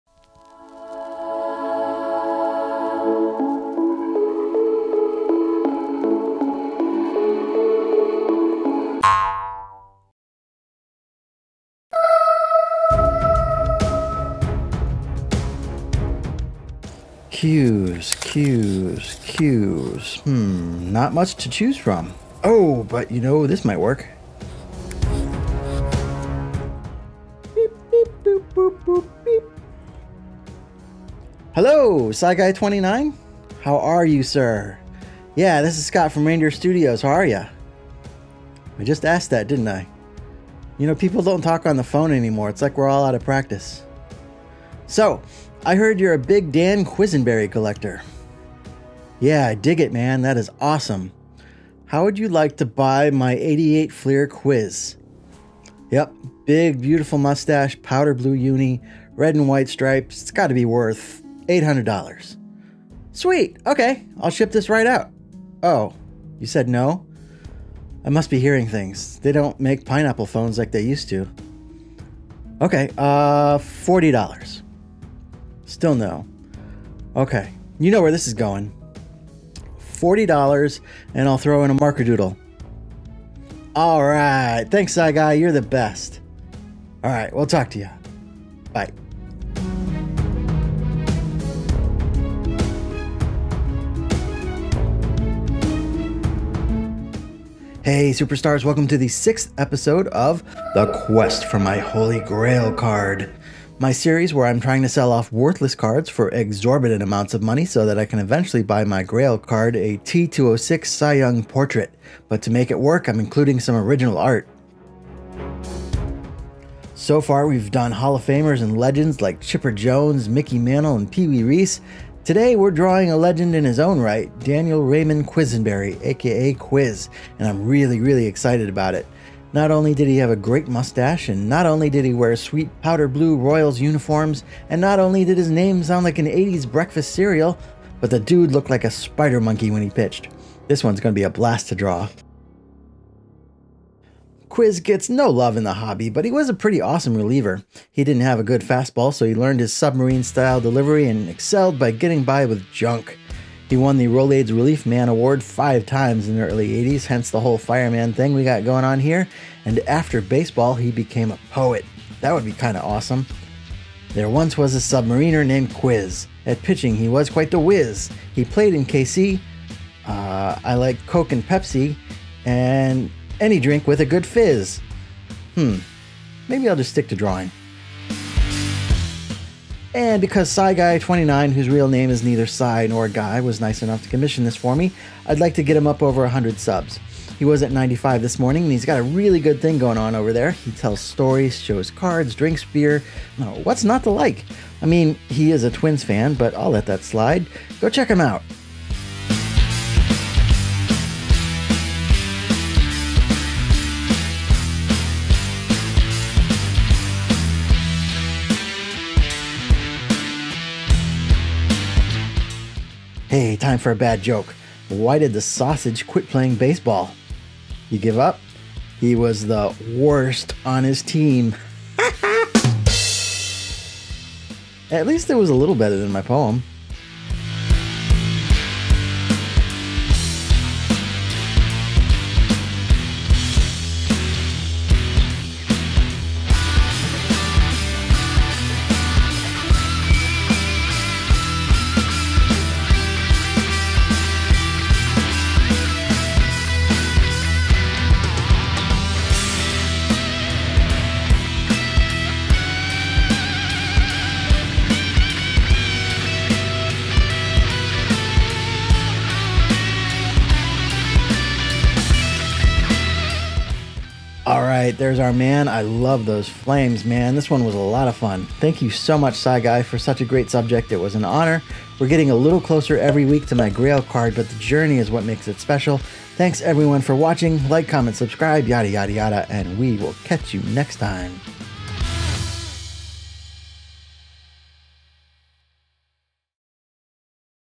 Dan Quisenberry Marker Doodle for CyGuy29 | Quest for my Holy Grail
This is my new series called Quest for my Holy Grail Card. In each episode, I’ll sell a card for an exorbitant amount of money and throw in some art. My goal is to eventually earn enough to buy a T206 Cy Young portrait.

Today we’re doing a marker doodle of Dan Quisenberry for CyGuy29